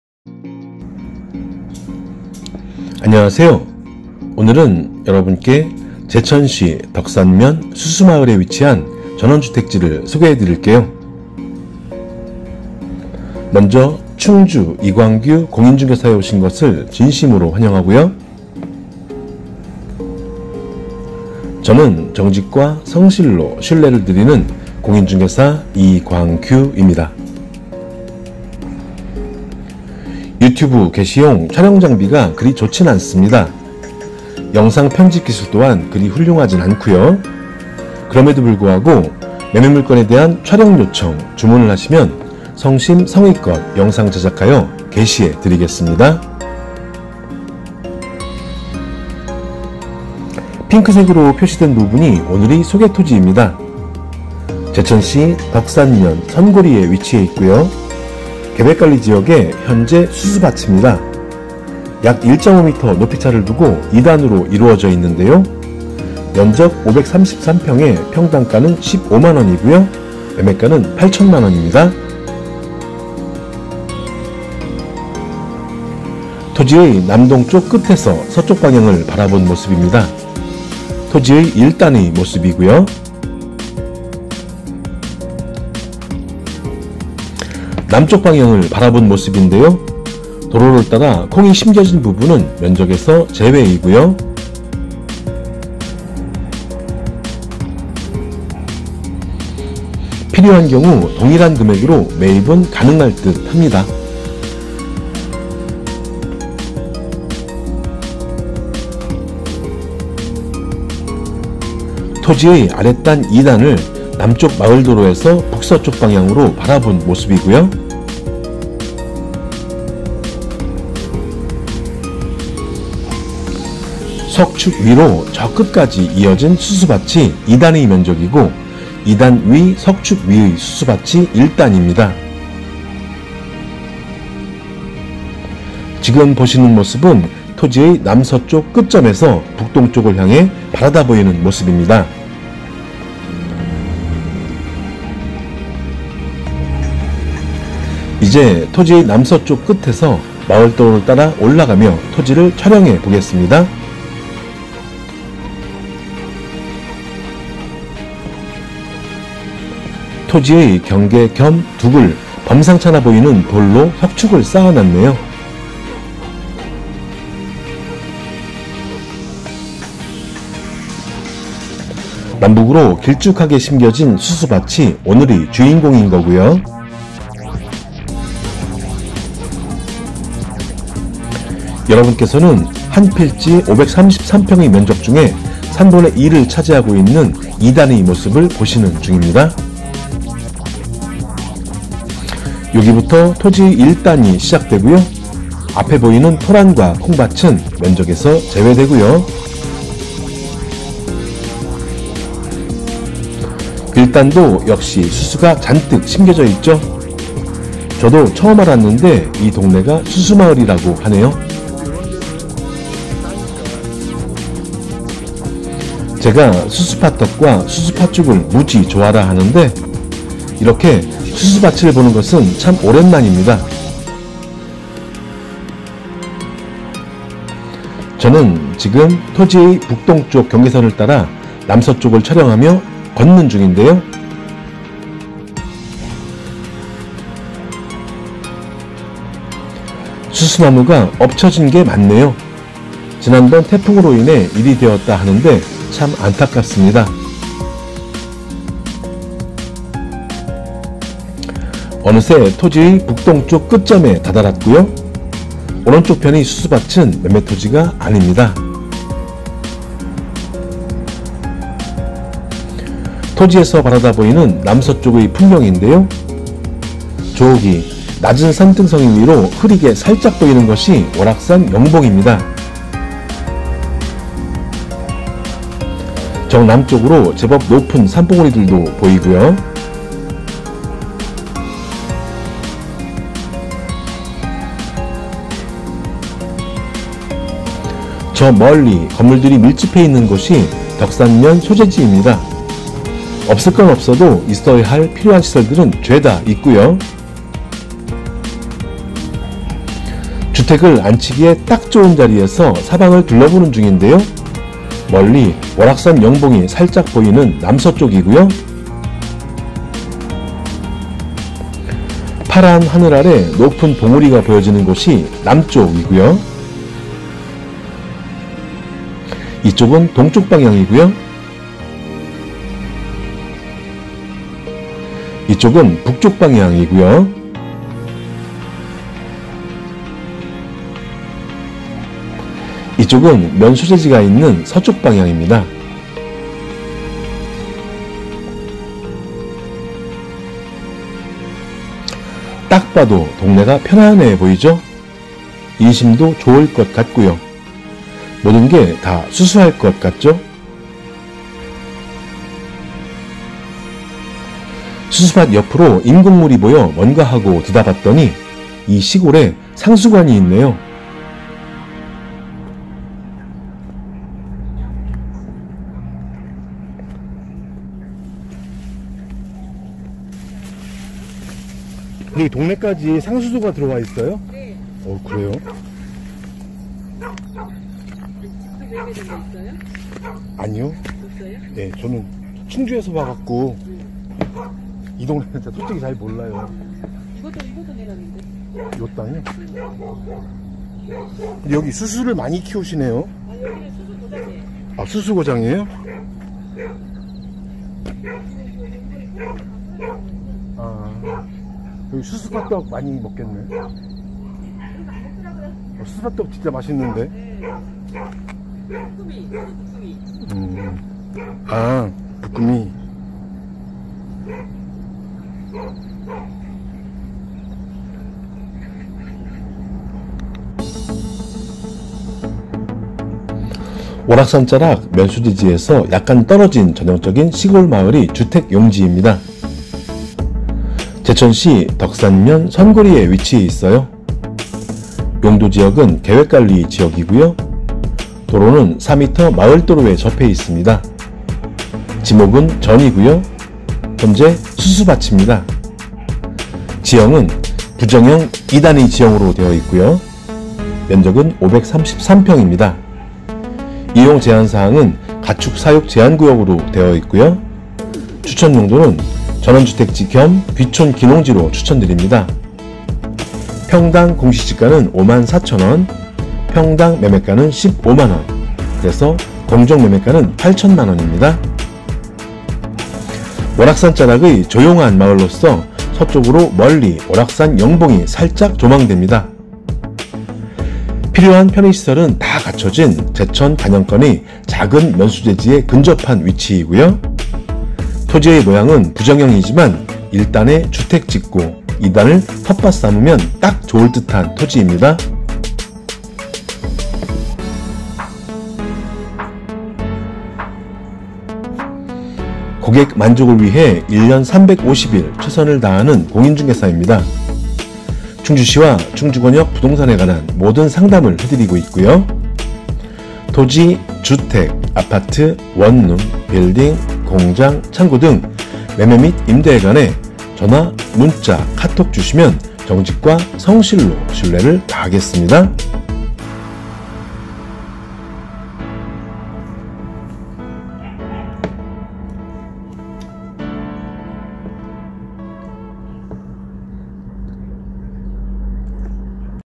[부동산] 전원주택용지 이광규166] 제천시 덕산면 선고리 월악산자락 수수마을 전 553평 매 8천 남서쪽으로 아주 멀찌감치 살짝 월악산 영봉 조망
【월악산자락 면소재지에서 약간 떨어진 전형적인 시골마을의 주택용지】
소재지: 제천시 덕산면 선고리
용도지역: 계획관리
도로: 4m도로
지목: 전
사용: 수수밭
지형: 부정형 2단 지형
면적: 533평
이용제한사항: 가축사육제한구역
추천용도: 전원주택지 겸 귀촌귀농지
평당공시가: 54,546
평당매매가: 150,000
공정매매가: 80,000,000
 ※ "월악산자락의 조용한 마을로써 서쪽으로 멀리 월악산 영봉이 살짝 조망
필요 편의시설은 다 갖춰진 제천 단양권의 작은 면소재지에 근접한 위치
토지의 모양 부정형이지만 1단에 주택짓고 2단을 텃밭삼으면 딱 좋을 듯"
『 충주 이광규 공인 ^ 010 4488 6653 』